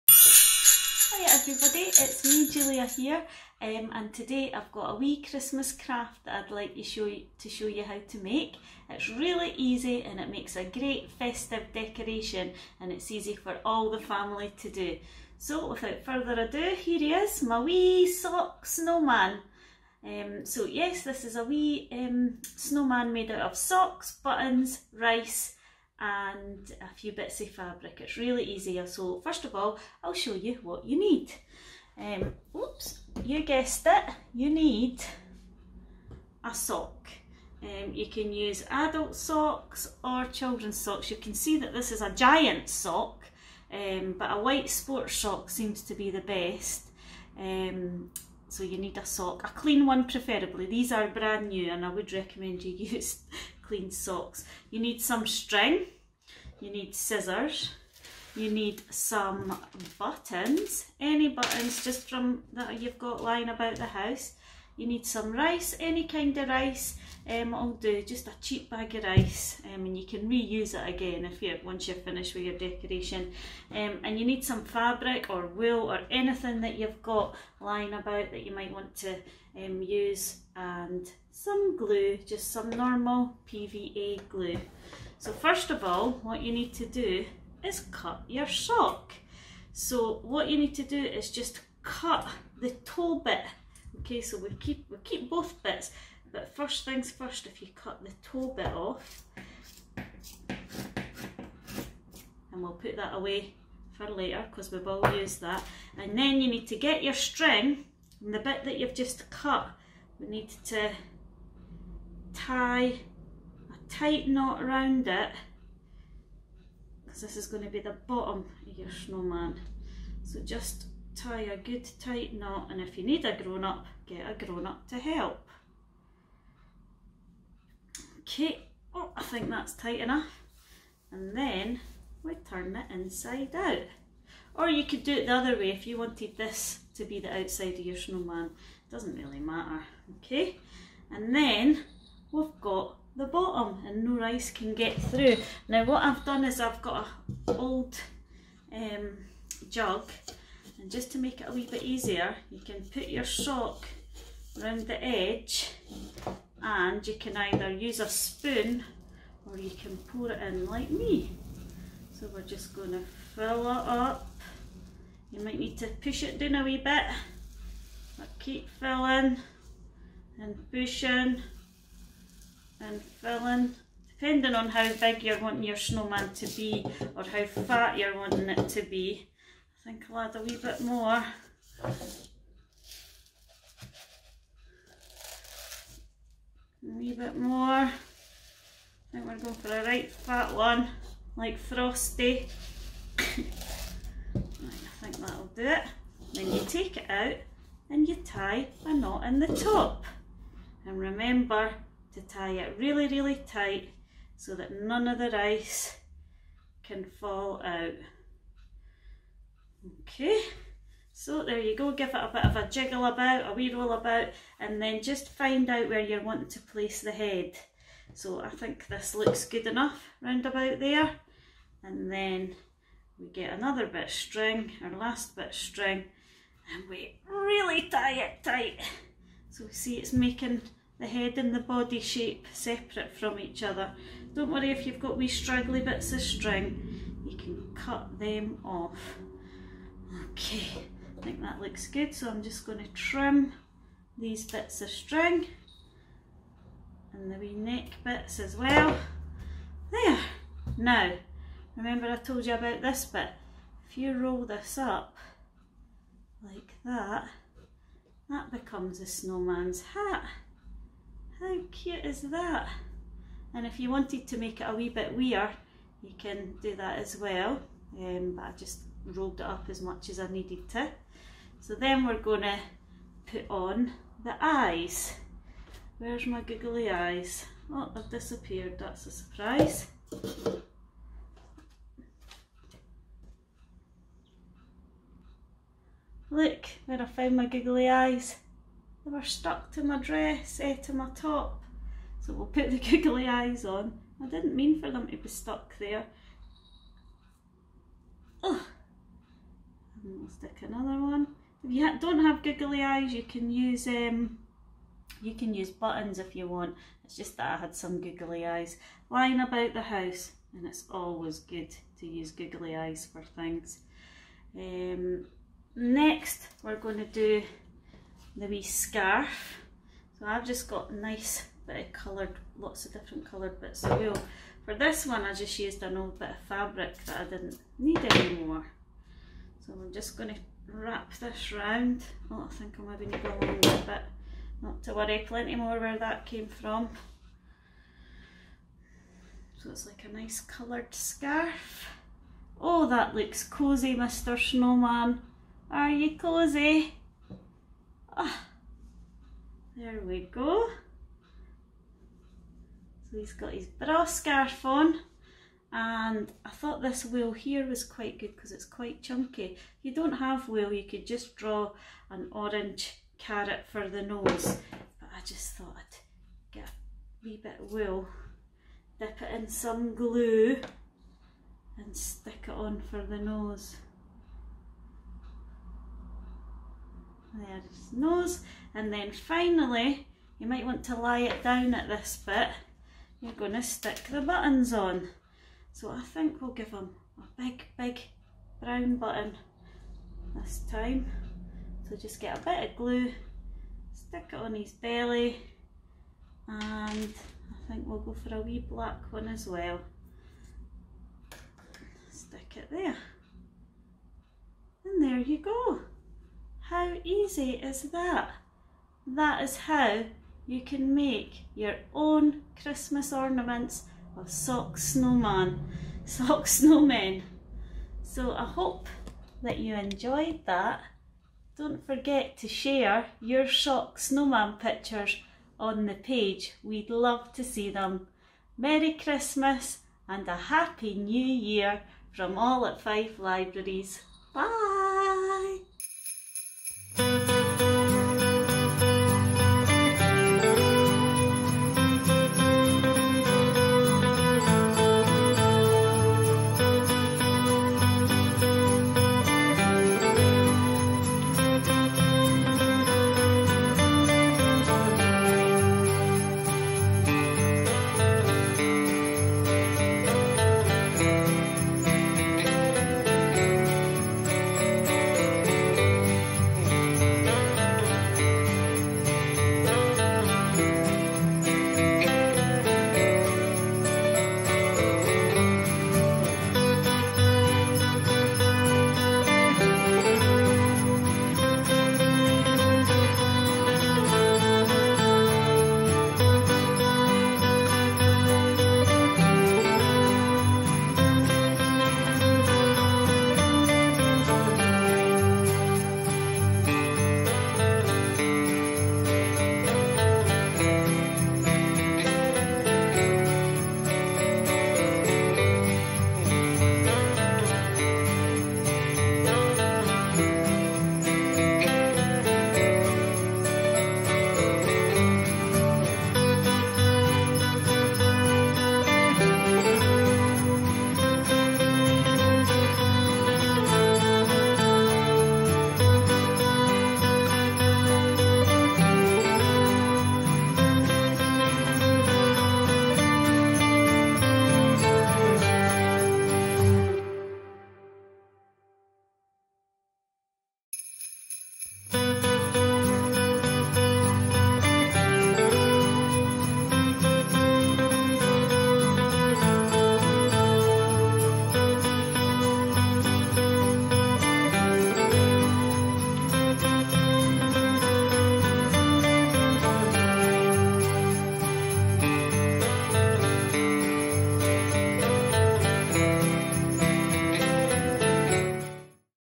OnFife Advent Calendar 11 - Sock Snowman Crafts and Mince Pies
A special double bill today with a crafts special from Julia and a mince pie recipe from Lisa!